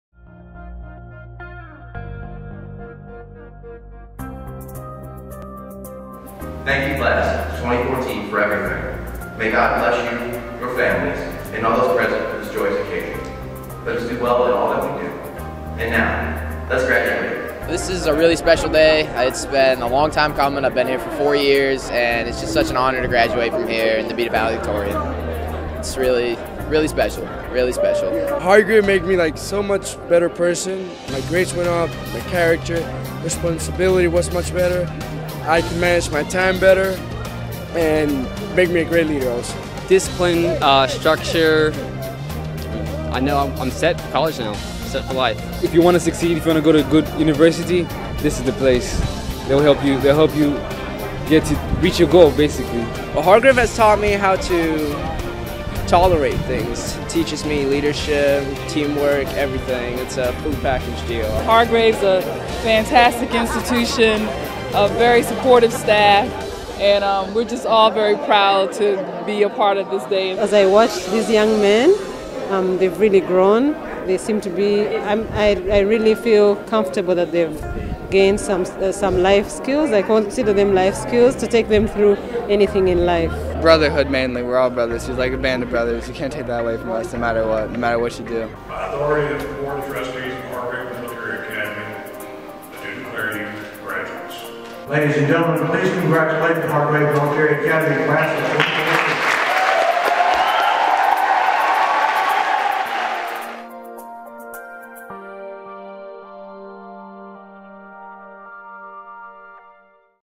Hargrave Military Academy's Class of 2014
On Saturday, May 17, 2014, Hargrave Military Academy's Class of 2014 walked across the stage, accepted their diplomas and became members of Hargrave's alumni.